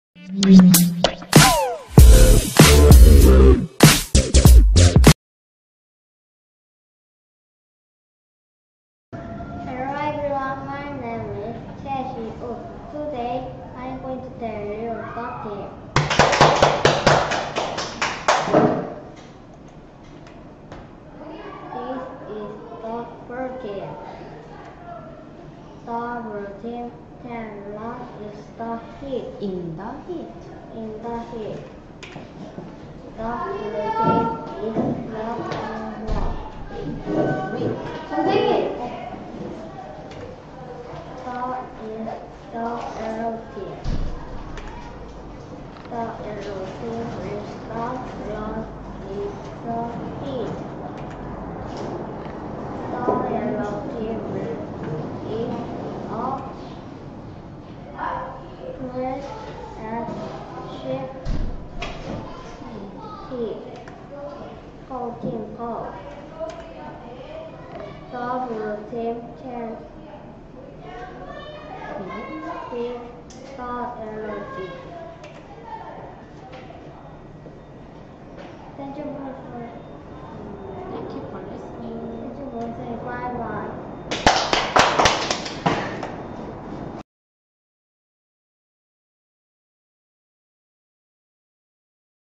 공터 송화7센터 201804 발표회 최시우
시우의 GK002 첫발표!
스토리가 훨씬 길어졌는데도 너무 너무 잘했어요^^